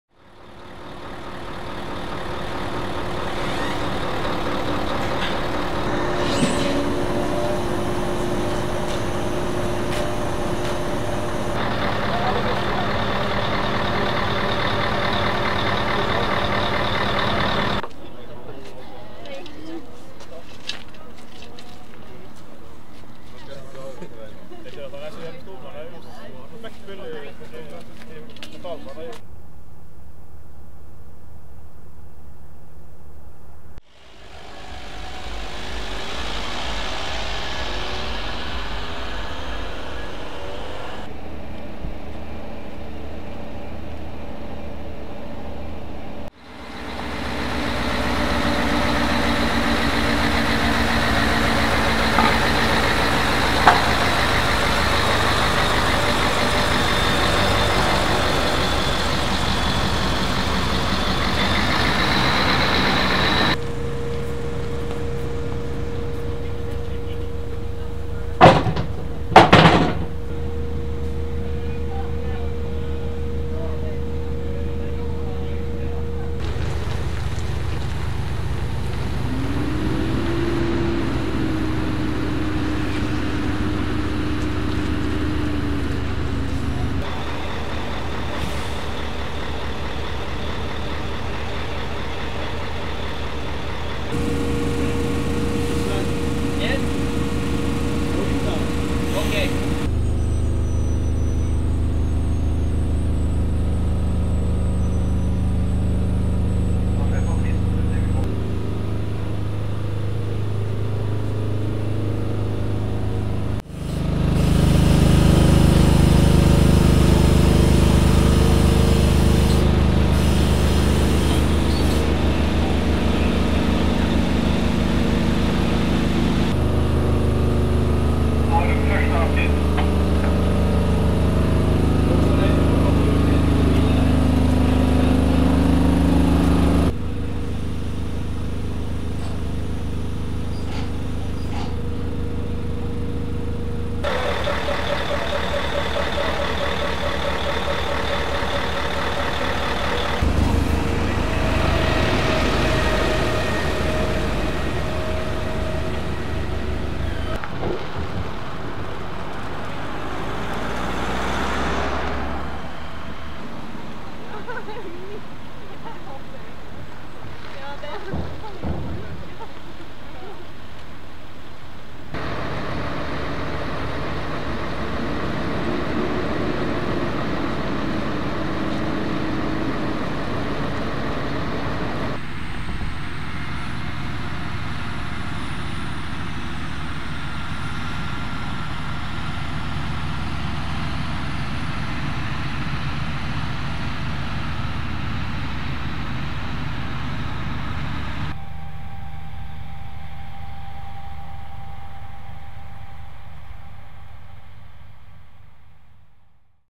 Transformatortransport
Flytting av ei transformator frå Sandane til Moskog, oppdrag for NRK Sogn og Fjordane, 27.april 2013.